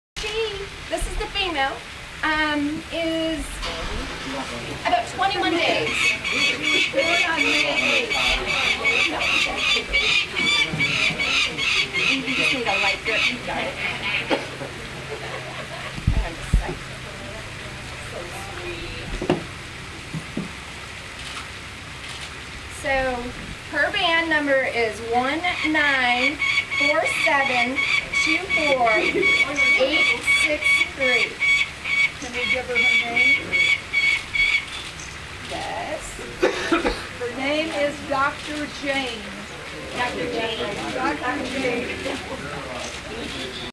Falcon chicks banded at the University of Toledo
Naturalists from the Ohio Department of Natural Resource's Division of Wildlife band the 2014 University of Toledo peregrine falcon chicks. Tuesday, May 27, 2014